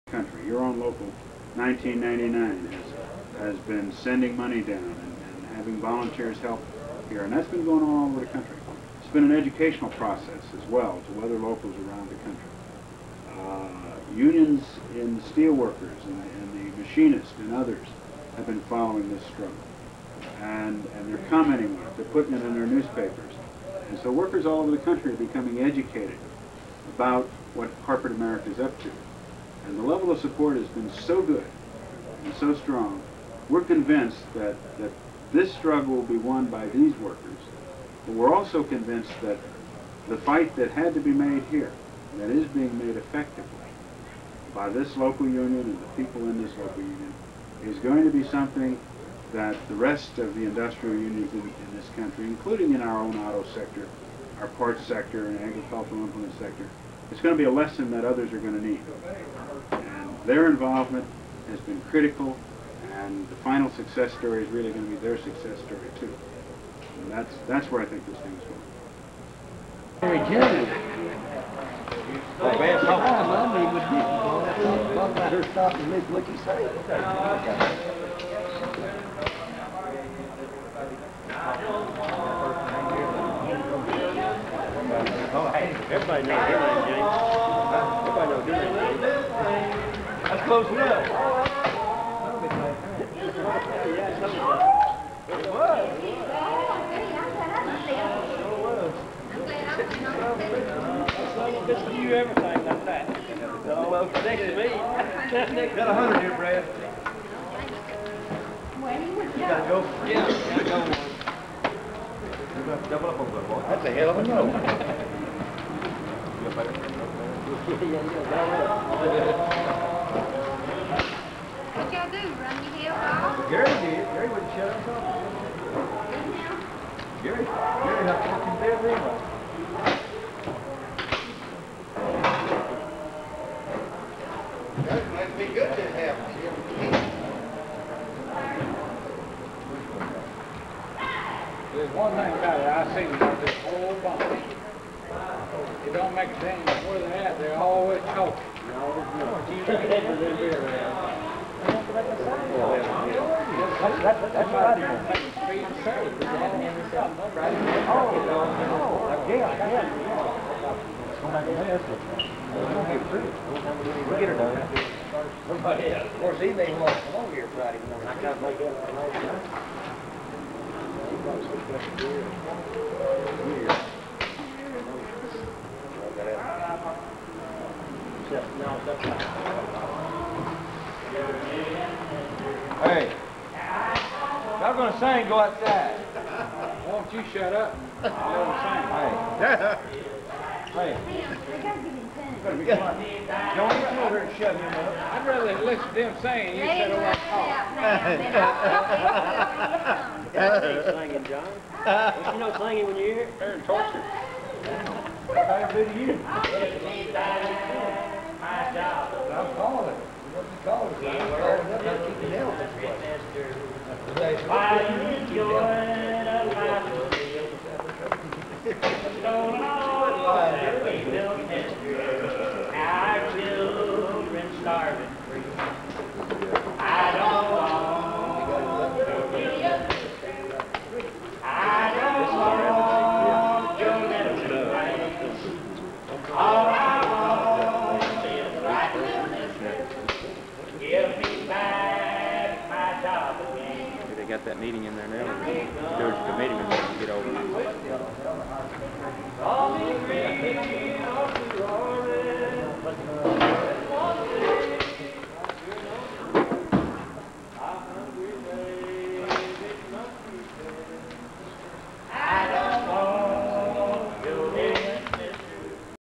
struggle20 WhatItAllMeans-Local 848's fight with LTV 1984-5
Explanations of the Union strategy for the contract fight